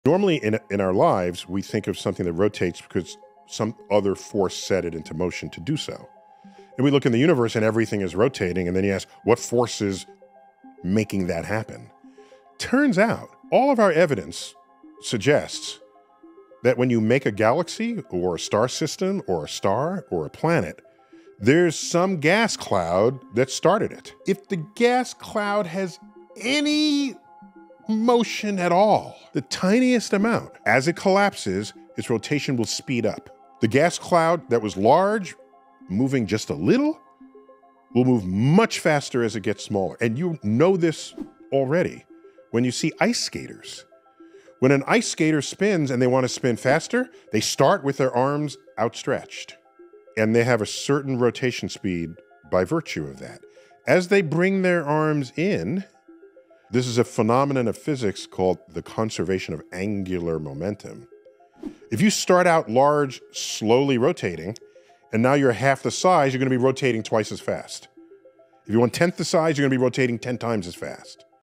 Why Galaxies Spin Like That (Galaxy Rotation Explained)
Explore the surprising reasons why galaxies spin! Ever wondered how a little movement in gas clouds can create huge rotations, similar to how ice skaters twirl faster by pulling their arms in? Dive into the world of angular momentum and see how everything in the universe is connected through motion.